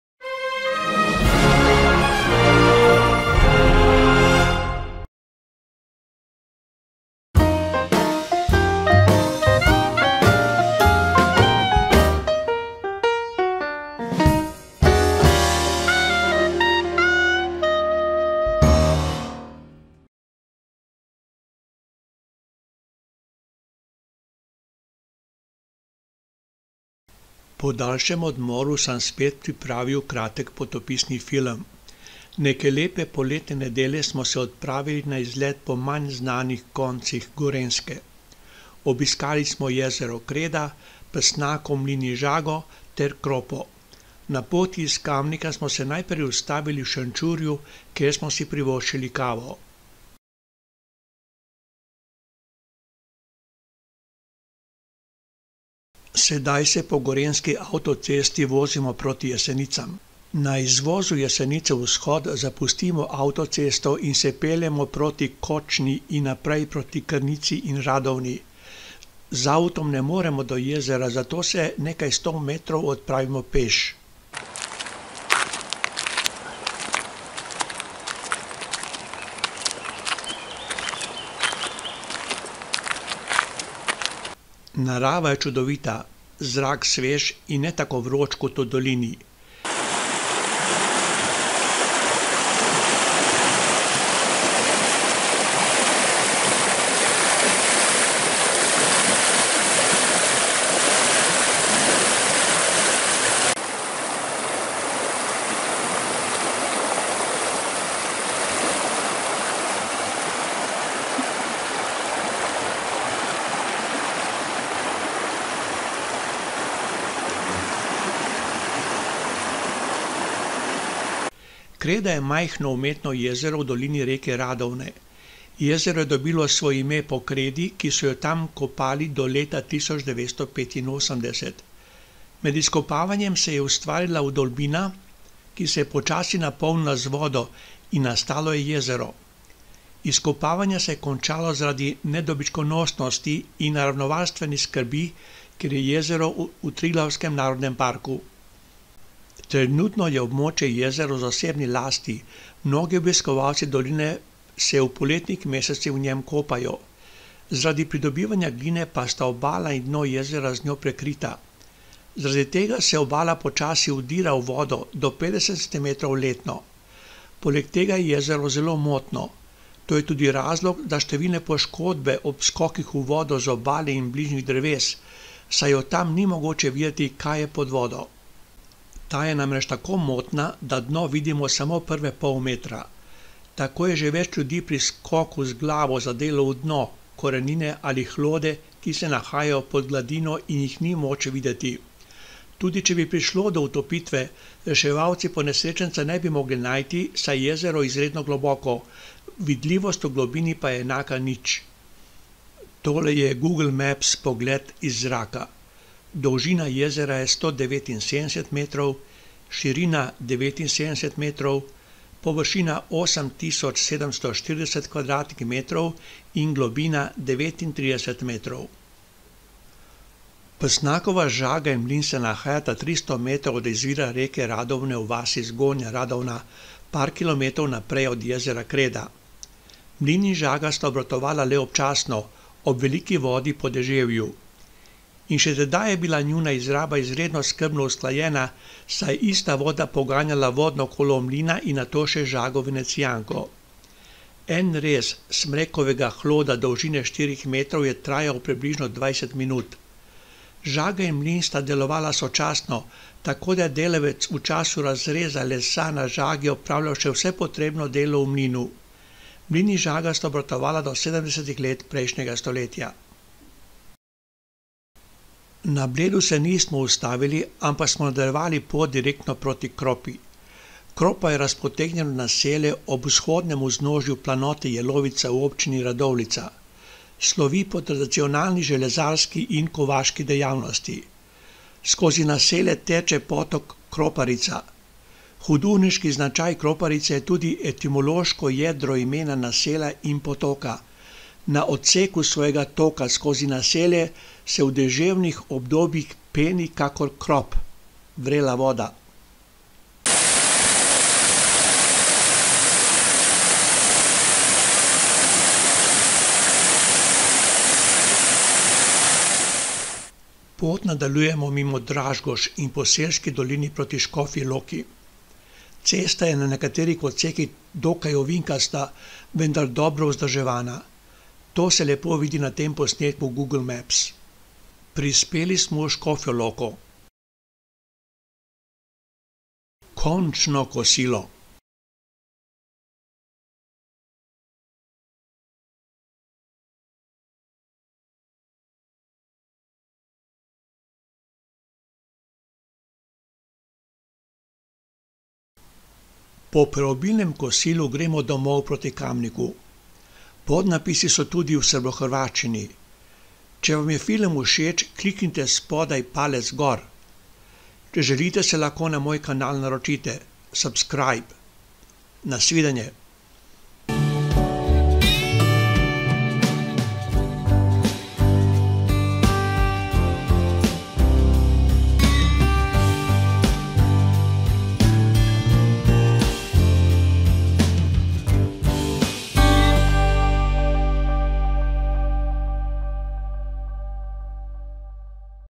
Manj znana Gorenjska
Kreda je majhno umetno jezero v Dolini reke Radovne. Jezero je dobilo svoje ime po kredi, ki so jo tam kopali do leta 1985. Med kopanjem se je ustvarila vdolbina, ki se je počasi napolnila z vodo in nastalo je jezero. Izkopavanje se je končalo zaradi nedobičkonosnosti in naravovarstvenih skrbi ker je jezero je v Triglavskem narodnem parku, jezero pa je ostalo. Trenutno je območje jezera v zasebni lasti, mnogi obiskovalci doline se v poletnih mesecih v njem kopajo. Zaradi kopanja gline pa sta obala in dno jezera z njo prekrita. Zaradi tega se obala počasi vdira v vodo. Do 50 cm letno. Poleg tega je jezero zelo motno. To je tudi razlog za številne poškodbe ob skokih v vodo z obale in bližnjih dreves, saj iz tam ni mogoče videti kaj je pod vodo. Ta je namreč tako motna da dno vidimo samo prve pol metra. Tako je že več ljudi pri skoku z glavo zadelo v dno, korenine ali hlode ki se nahajajo pod gladino in jih ni moč videti. Tudi če bi prišlo do utopitve na primer, reševalci ponesrečenca ne bi mogli najti saj je jezero izredno globoko, vidljivost pa je enaka nič. Pozimi jezero zamrzne, zato je na njem moč drsati.
Dolžina jezera je 179 m, širina 79 m, površina 8.740 m2 in globina 39 m.
Psnakova žaga in mlin se nahajata 300 metrov od izvira reke Radovne v vasi Zgornja Radovna, par kilometrov naprej od Jezera Kreda. Mlin in žaga sta delovala le občasno - ob veliki vodi po deževju. In še tedaj je bila njuna izraba izredno skrbno usklajena, saj je ista voda poganjala vodno kolo mlina in nato še žago "venecijanko". En rez smrekovega hloda dolžine štirih metrov je trajal približno 20 minut. Žaga in mlin sta delovala sočasno, tako da je delavec v času razleza lesa na žagi opravljal še vse potrebno delo v mlinu.  Mlin in žaga sta obratovala do 70-ih let prejšnjega stoletja.
Kropa je razpotegnjeno naselje ob vzhodnem vznožju planote Jelovica v Občini Radovljica. Slovi po tradicionalni železarski in kovaški dejavnosti. Skozi naselje teče potok Kroparica. Hudourniški značaj Kroparice je tudi etimološko jedro imena naselja in potoka: na odseku svojega toka skozi naselje se v deževnih obdobjih peni kakor »krop« (vrela voda).
(Wikipedia)
Zračni posnetki so Google Maps.